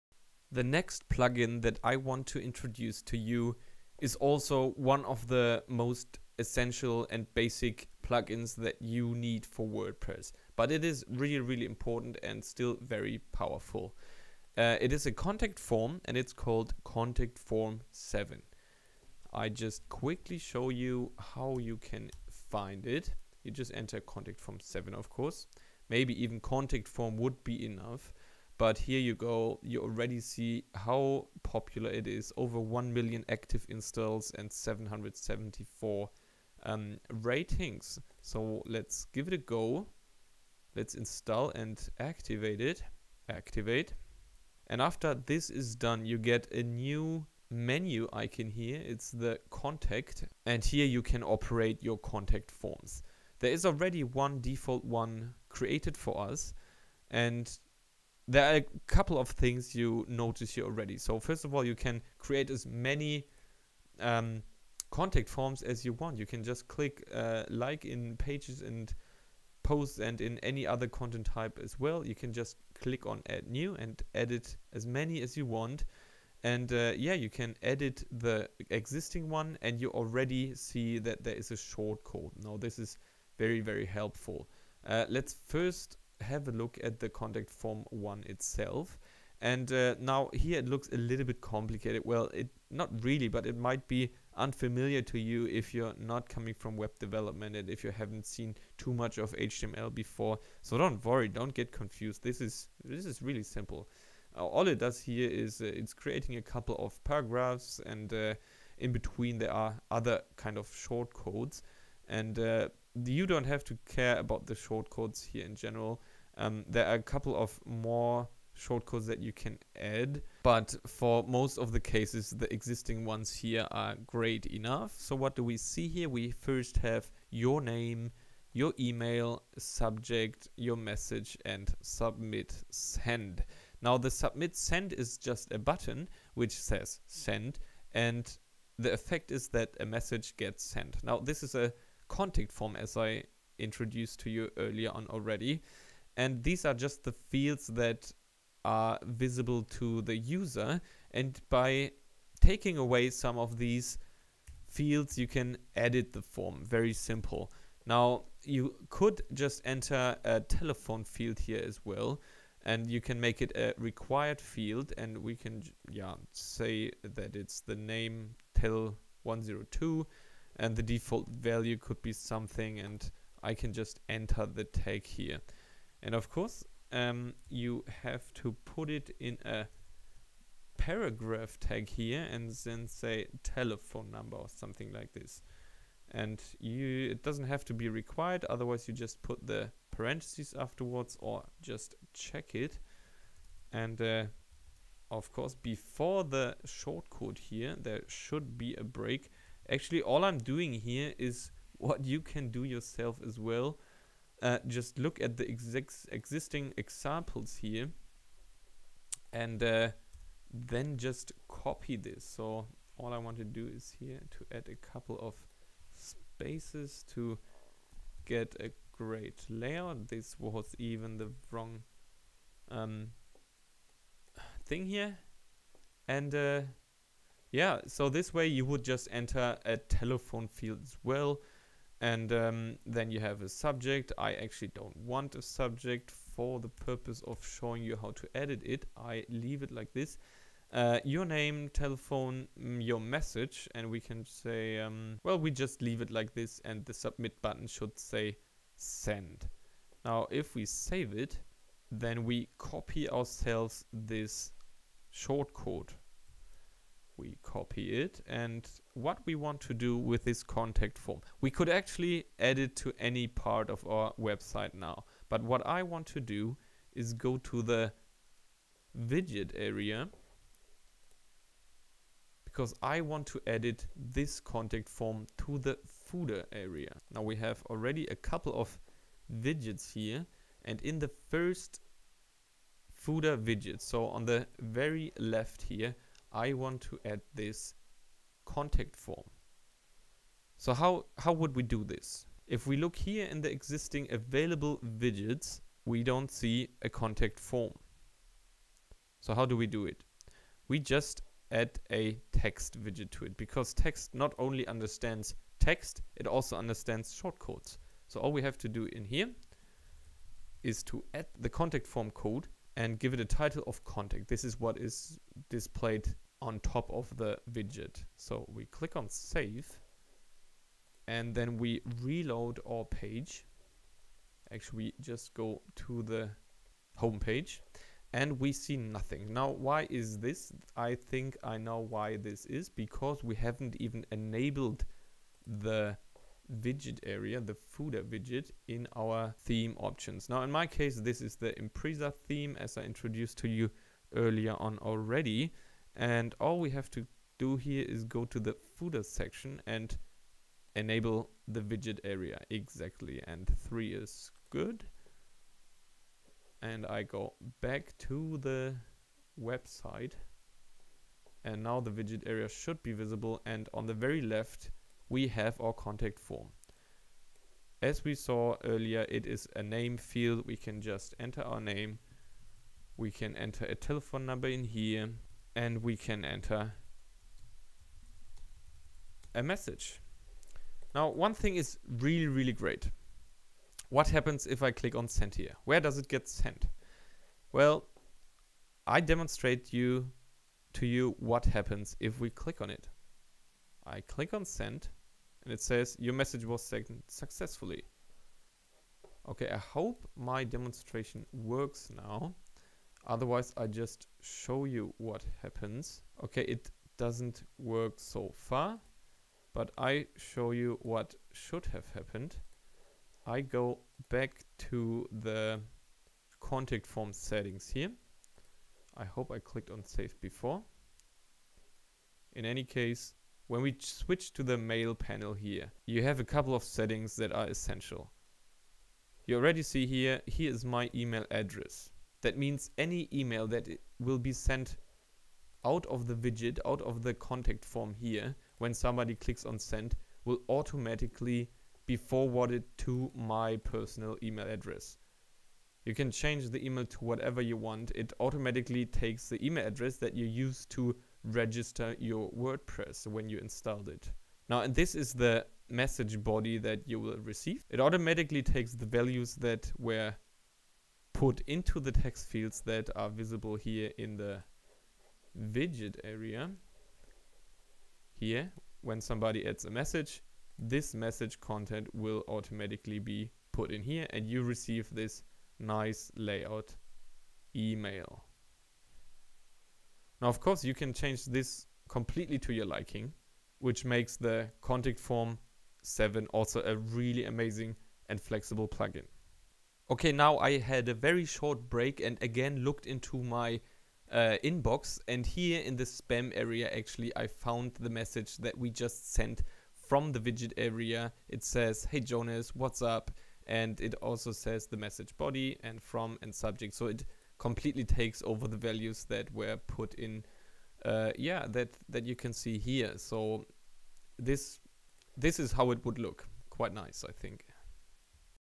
Contact Form 7 WordPress Tutorial (English)
In this video you will learn how you can easily create a contact form for WordPress using the great free Plugin Contact Form 7

Yours Jonas Schindler